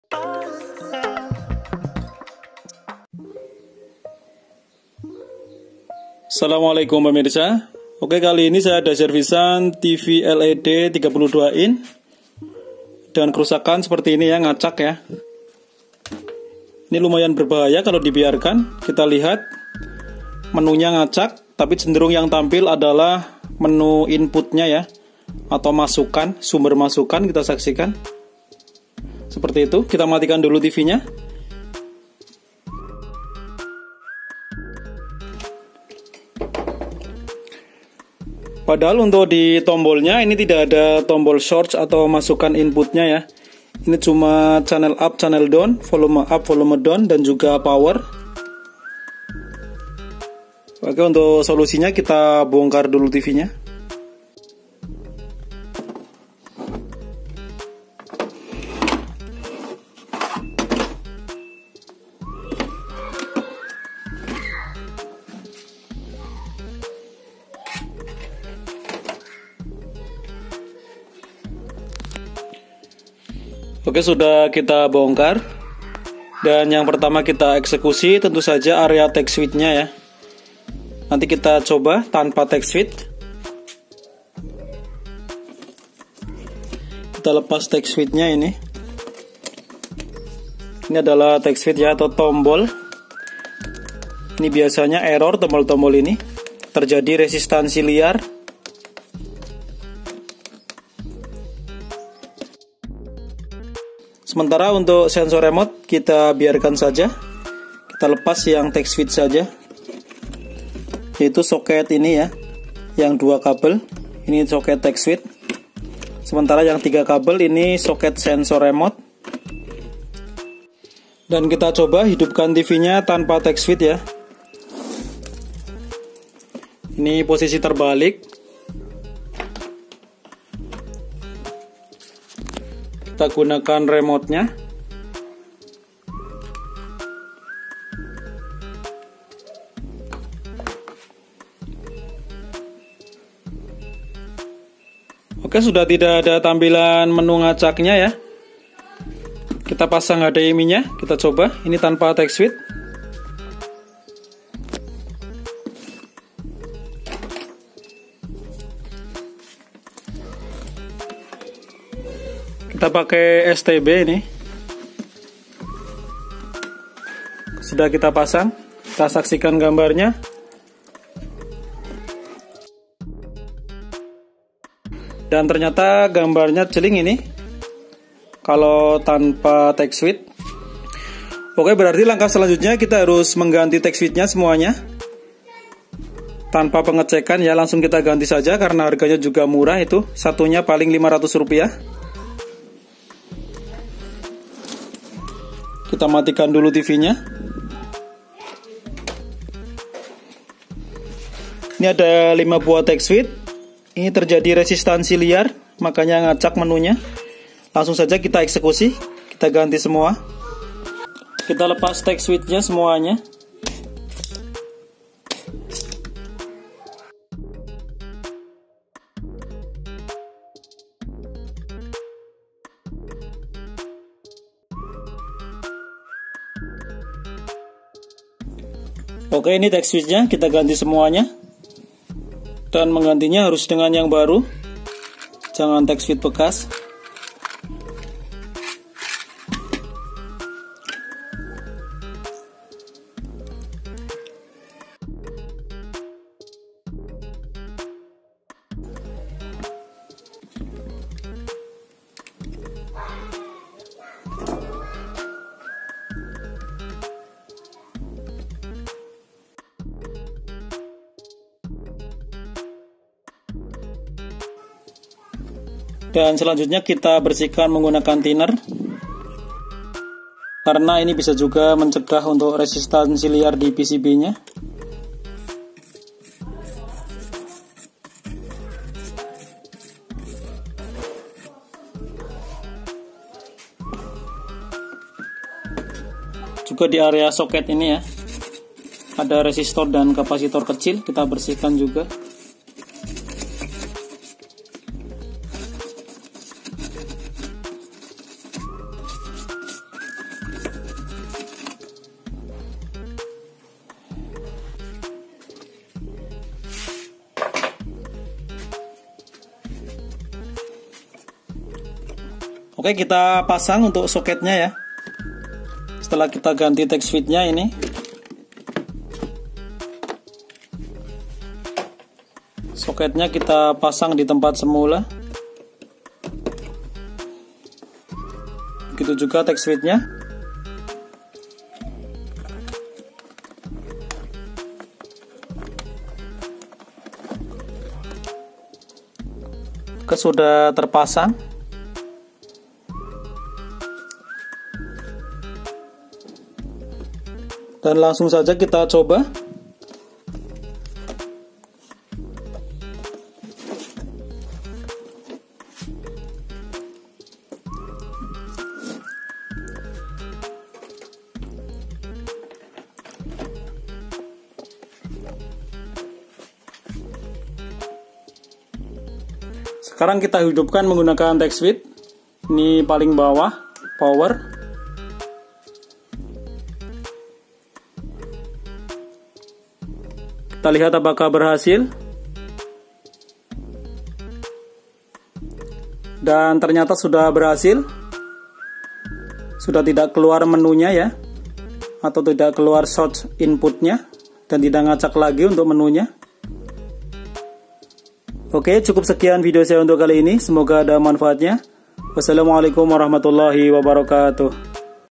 TV LED POLYTRON TAMPILAN MENU ERROR/NGACAK
video servis tv LED polytron dengan kerusakan tampilan menunya error/ngacak dan sering keluar menu sumber masukan atau input atau source,,,ternyata solusinya mudah,,,selamat menonton.